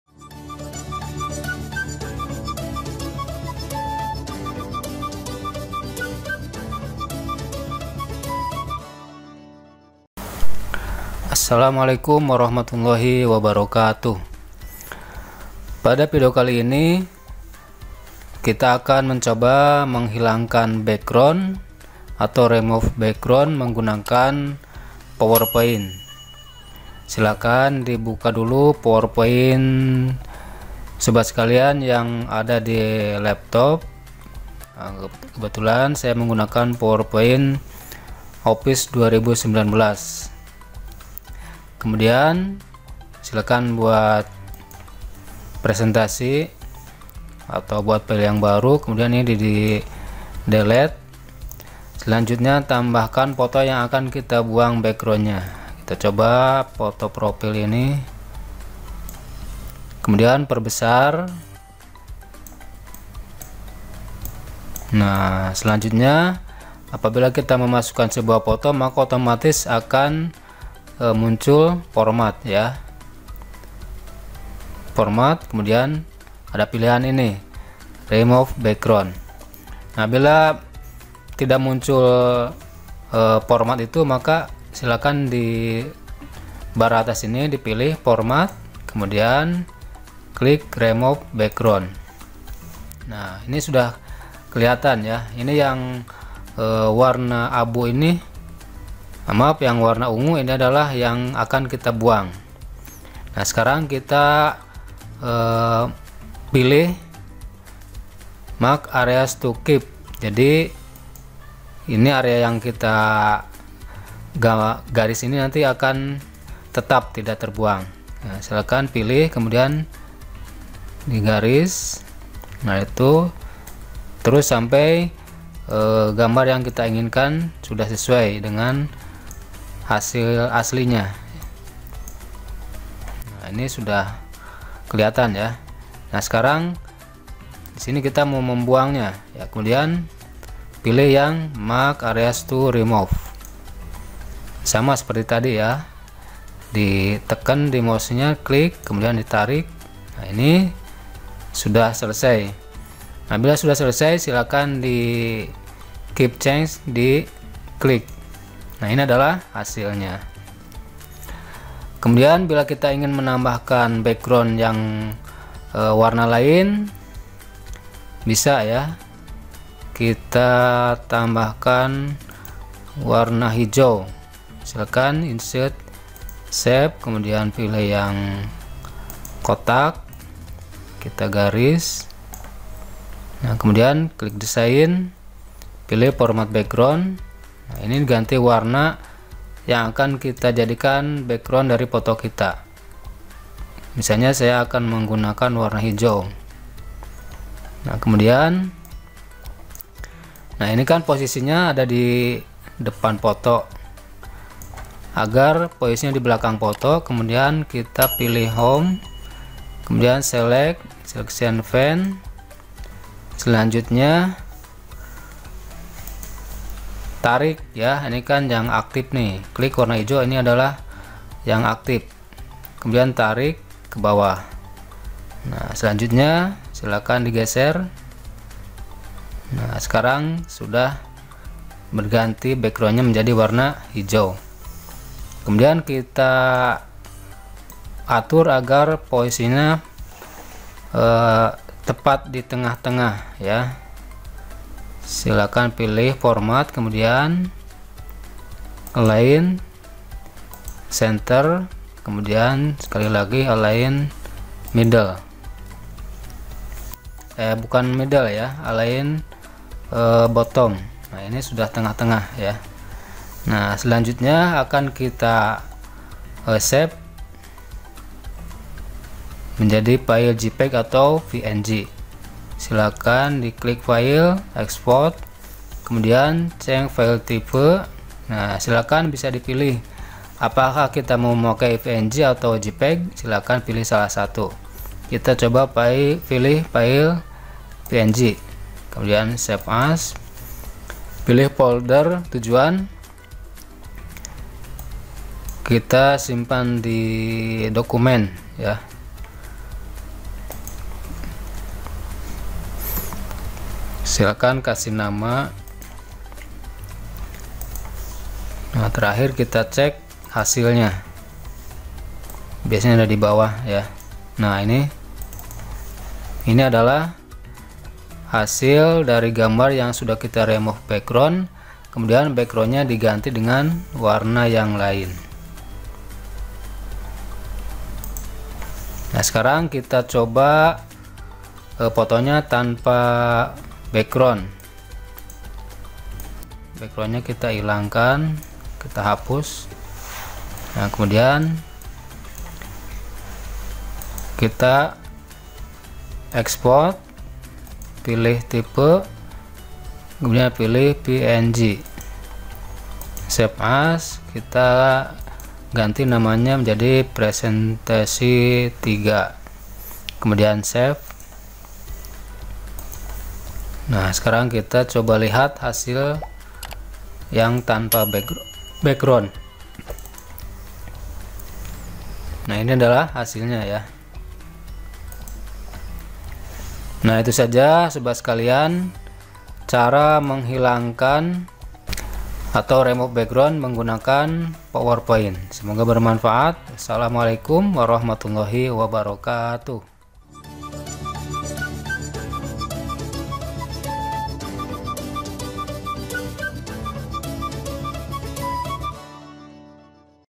Cara Remove Background dengan PowerPoint
1. Buka PowerPoint
2. Buat file baru/Presentation
3. Blok semua bagian pada layar kerja
4. Insert image atau drop & drag
5. Klik Format
6. Klik Remove background (pojok kiri atas)

Selanjutnya ikuti tutorialnya di video, jangan di-skip agar tidak ada yang kelewat.